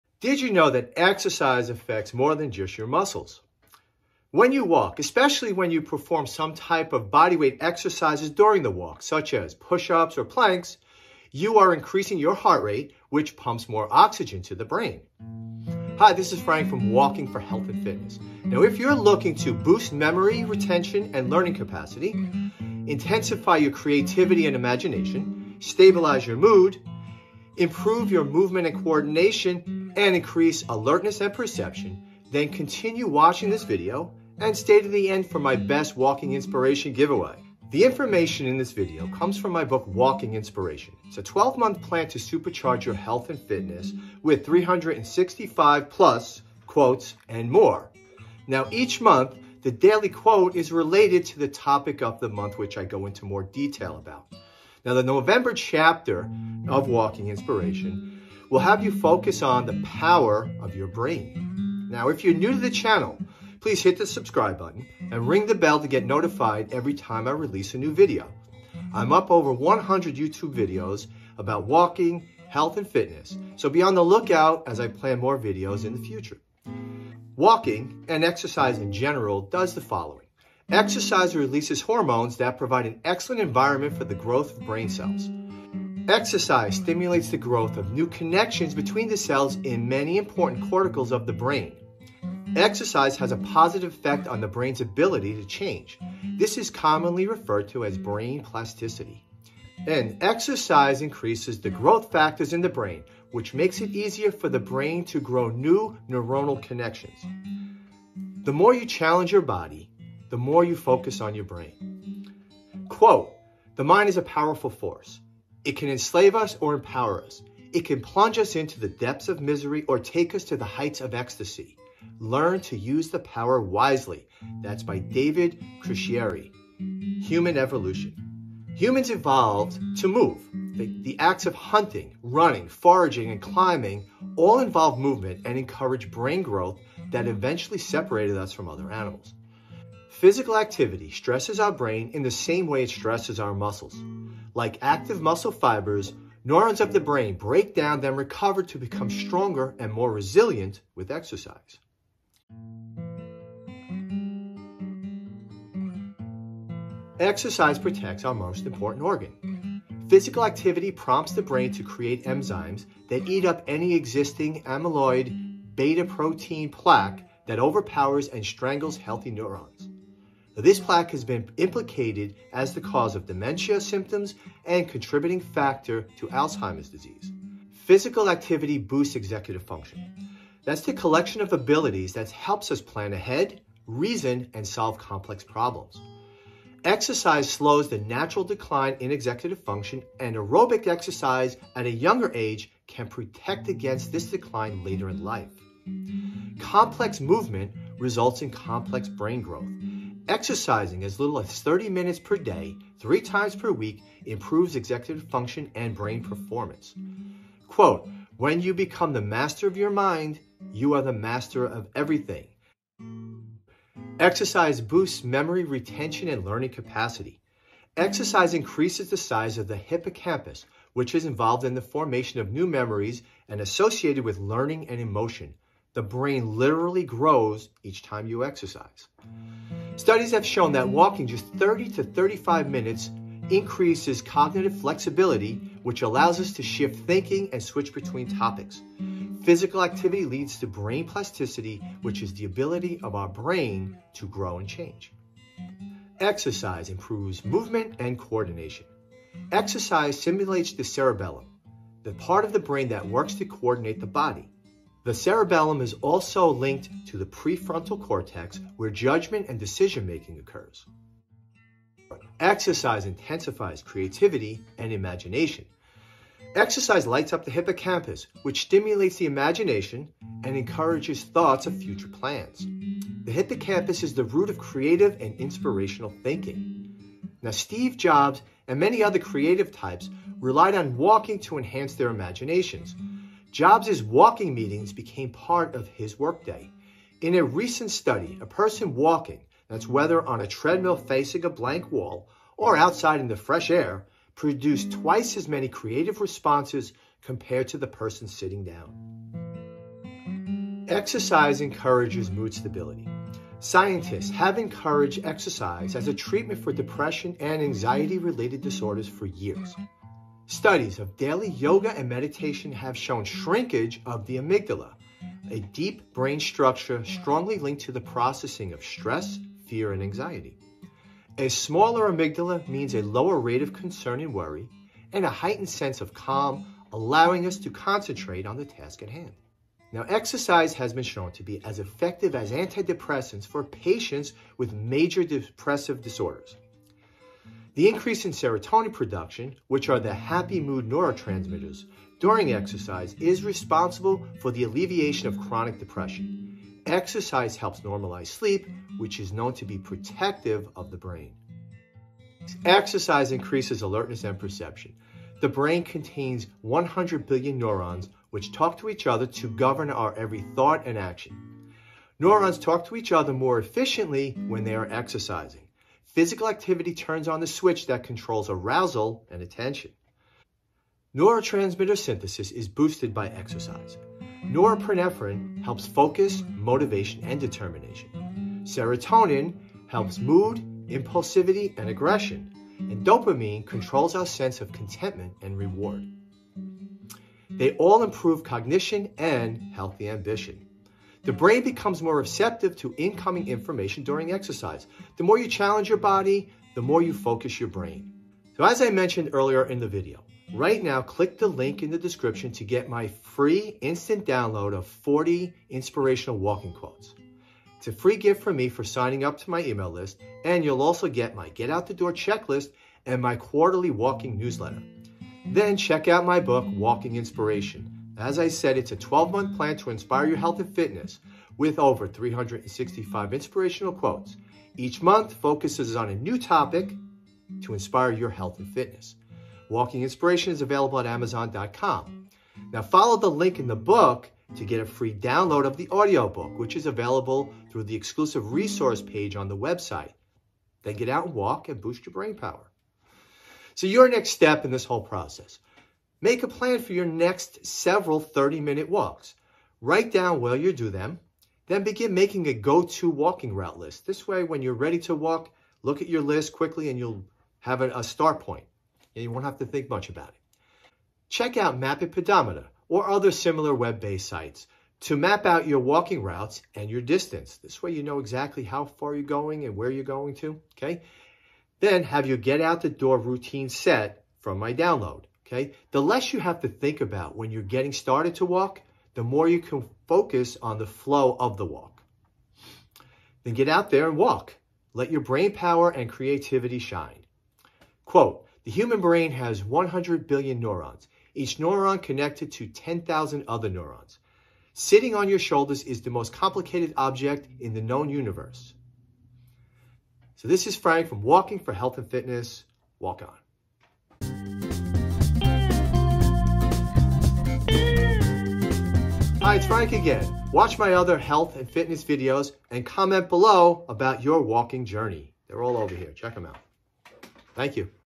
Seven Ways Walking and Exercise Boosts Brain Power
Walking Exercise and The Power of Your Brain

Looking to start walking for health and fitness but don't know how to start?

Blog Post: Coming Soon

Walking books by Frank S. Ring

Walking Programs by Frank S. Ring

1. Exercise Protects Our Most Important Organ 2:38
2. Boosts Executive Function 2:59
3. Exercise Boosts Memory Retention & Learning Capacity 3:36
4. Exercise Improves Movement & Coordination 4:08
5. Exercise Intensifies Creativity & Imagination 4:25
6. Exercise Encourages Mood Stability 5:02
7. Exercise Increases Alertness & Perception 5:55

They all improve cognition and healthy ambition.

The brain becomes more receptive to incoming information during exercise. The more you challenge your body, the more you focus your brain.

Did you know that exercise affects more than just your muscles?

When you walk, especially if you perform some type of bodyweight exercises during the walk such as pushups or plank, you are increasing your heart rate which pumps more oxygen to the brain.

Benefits of Walking:
• Boost Memory Retention & Learning Capacity
• Intensify your Creativity & Imagination
• Stabilize your mood
• Improved your Movement & Coordination
• And, Increase Alertness & Perception

The information in this video comes from my Book Walking Inspiration: a 12-month plan to supercharge your health and fitness with 356+ quotes and more.

Each month, the daily quote is related the topic of that month.

In the November Chapter, you will focus on the the Power of Your Brain.

Background Guitar Music: Frank S. Ring